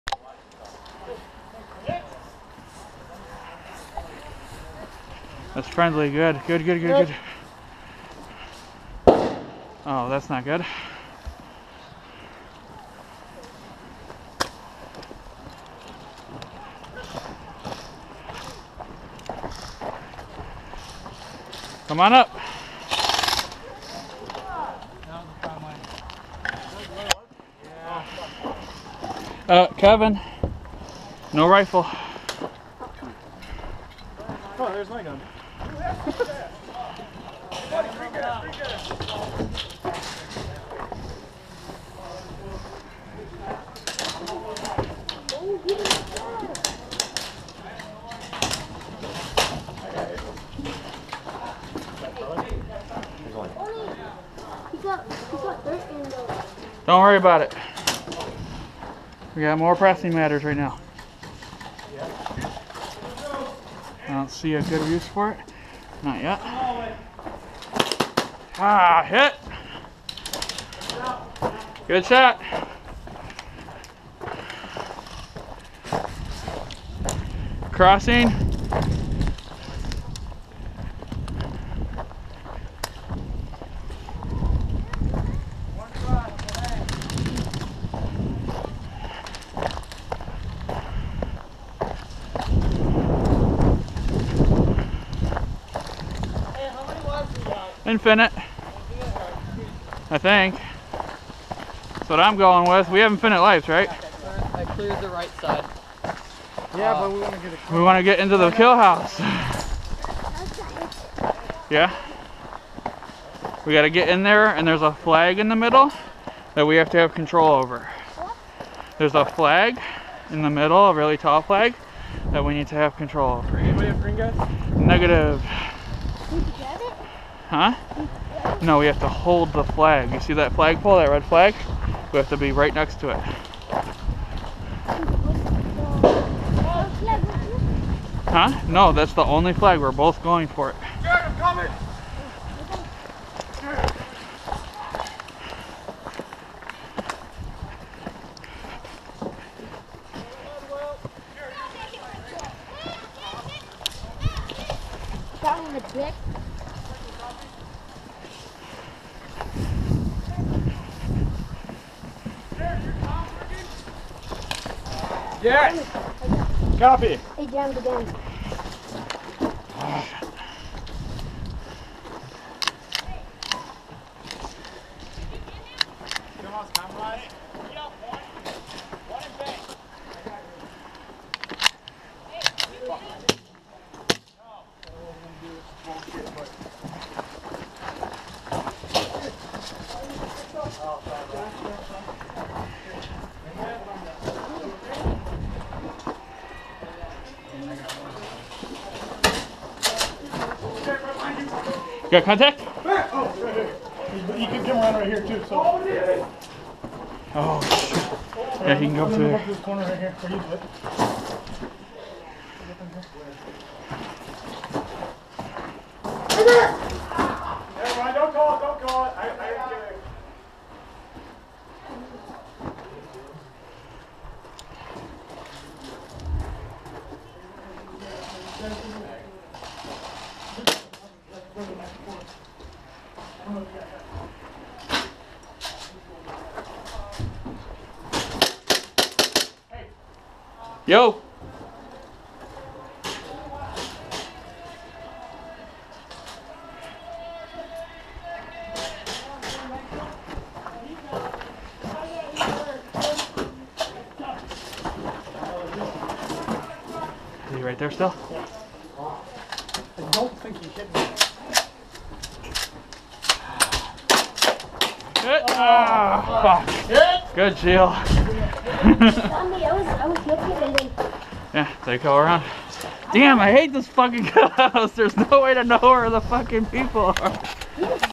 Black ops airsoft: kill house gameplay
Tony's footage from the kill house. Sorry the go pro was angled down, still got some decent shots inside the kill house.